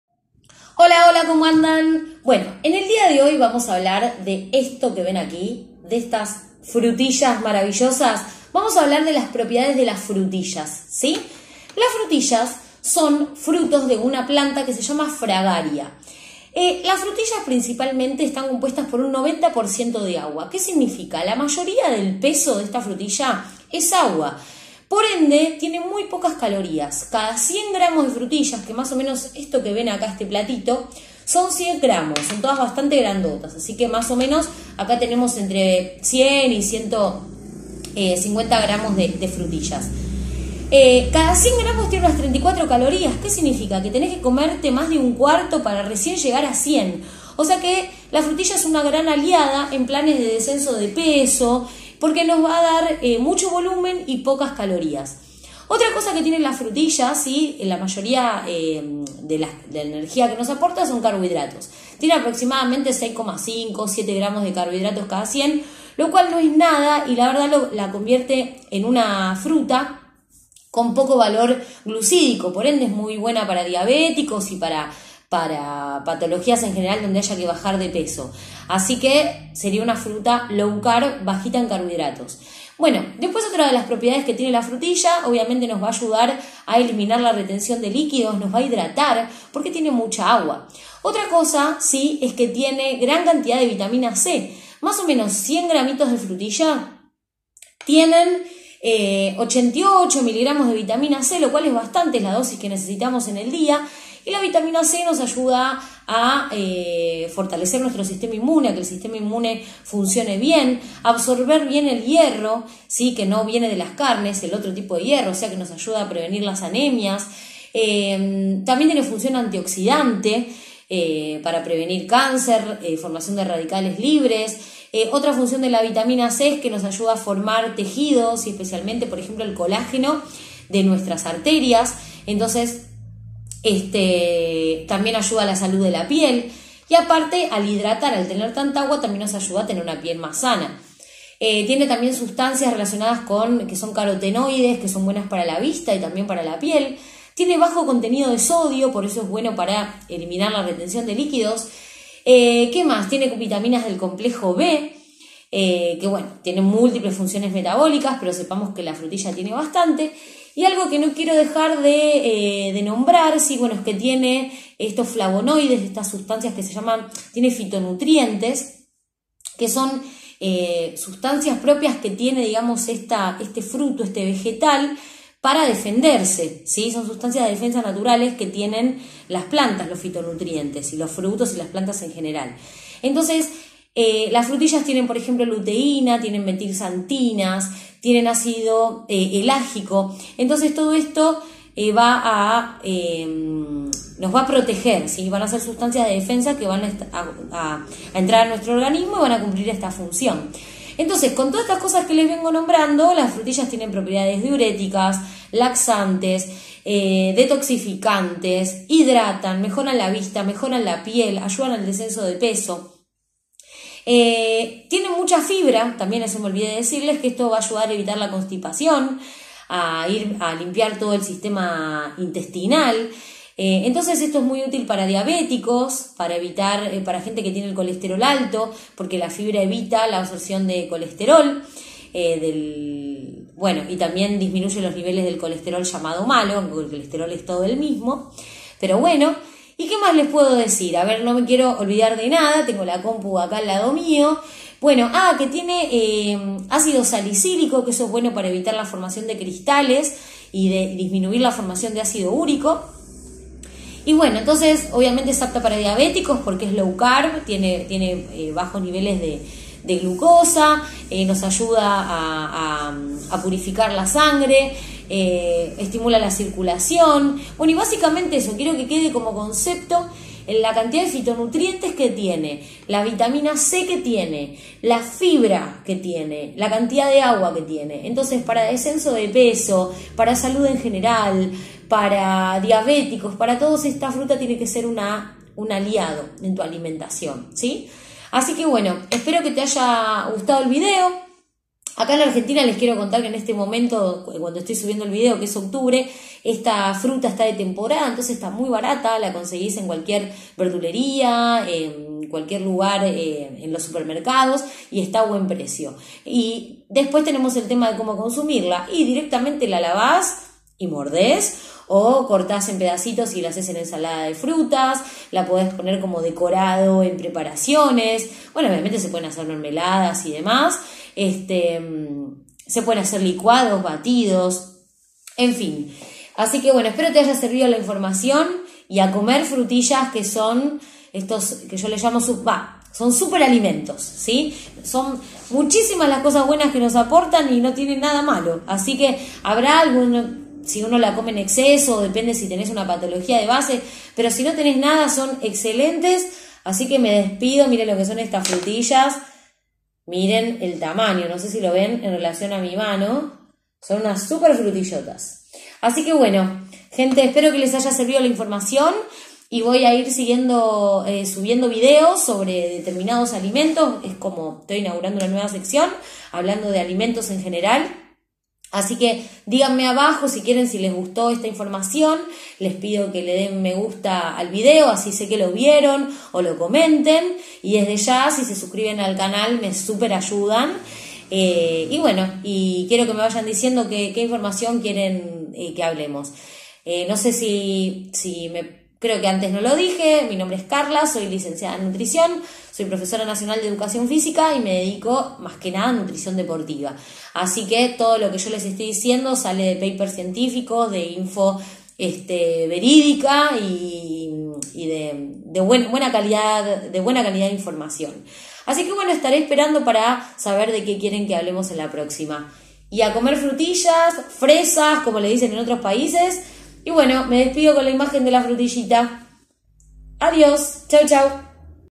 ATENCIÓN x MIRÁ LO QUE PASA SI COMÉS FRUTILLAS FRESAS Información Nutricional de las Frutillas
Qué Nutrientes nos aportan las Frutillas? Mirá todas las propiedades que tienen!

Lic. en Nutrición
MN 7117